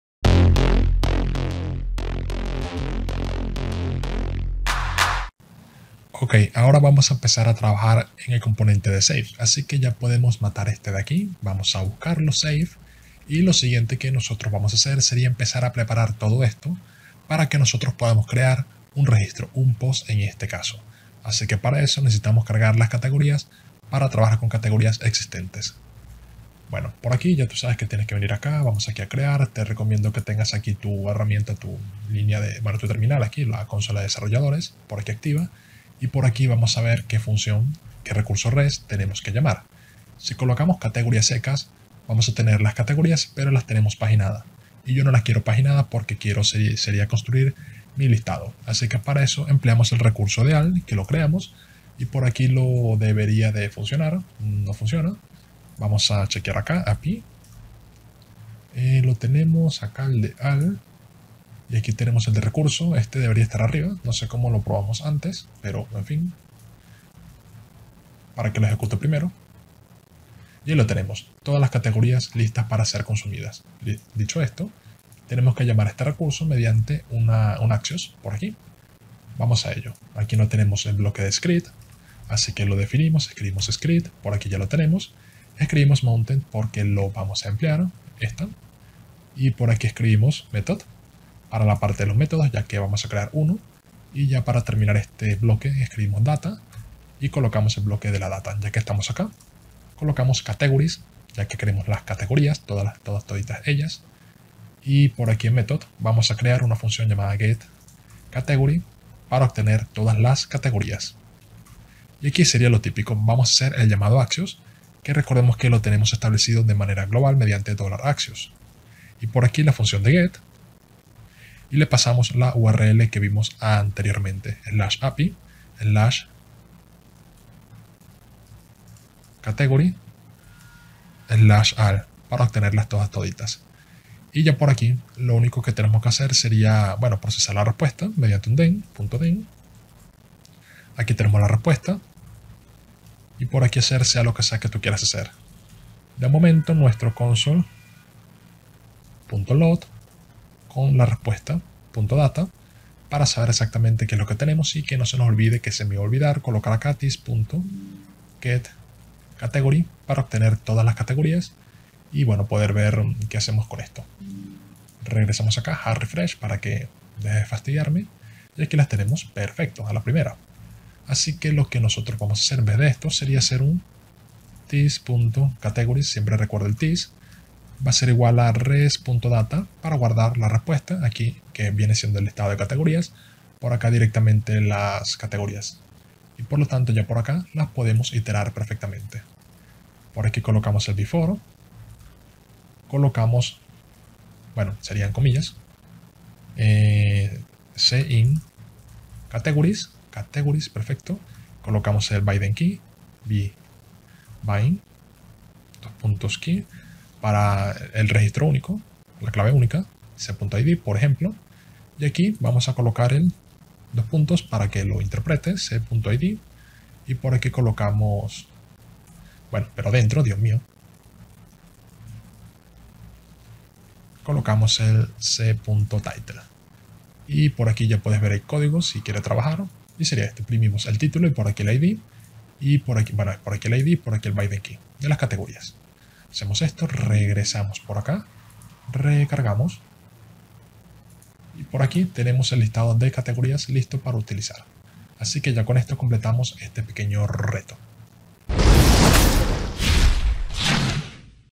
🔴 Curso Laravel - Cargar y popular un SELECT de categorías en Vue 10
🔴 Vamos a crear un listado para las categorías que usaremos más adelante para la creación de un POST.

Quieres una formación mas completa? prueba con mi curso COMPLETO!!!